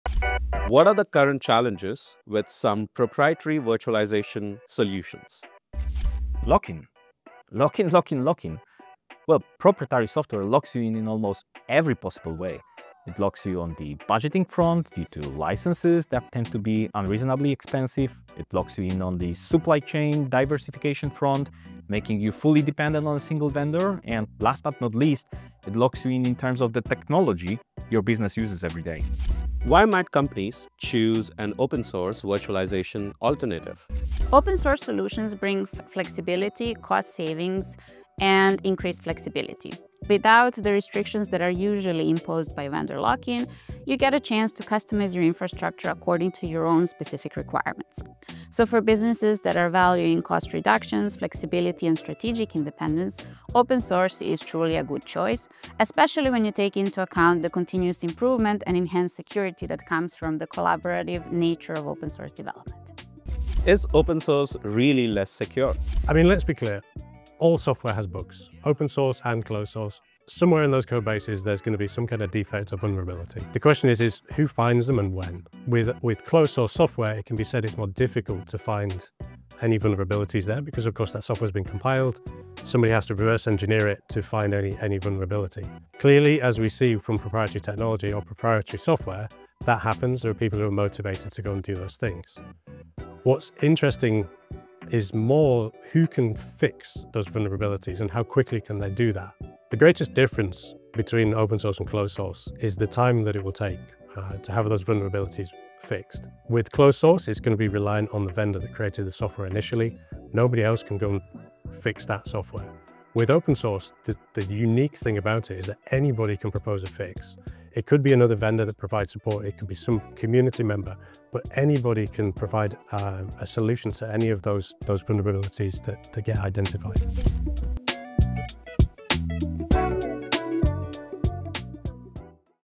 Open source virtualization: is it time to make the move?
What are the hidden costs of proprietary virtualization? In this episode, we unpack the challenges of vendor lock-in, from skyrocketing license fees to supply chain dependency and limited tech choices, and why open source alternatives offer businesses a smarter path forward.

We explore how open source virtualization delivers flexibility, cost savings, and strategic independence.

Timestamps
00:00 - The real impact of vendor lock-in in virtualization
00:28 - Why open source means freedom, savings, and flexibility
01:02 - Security myths: open source vs. closed source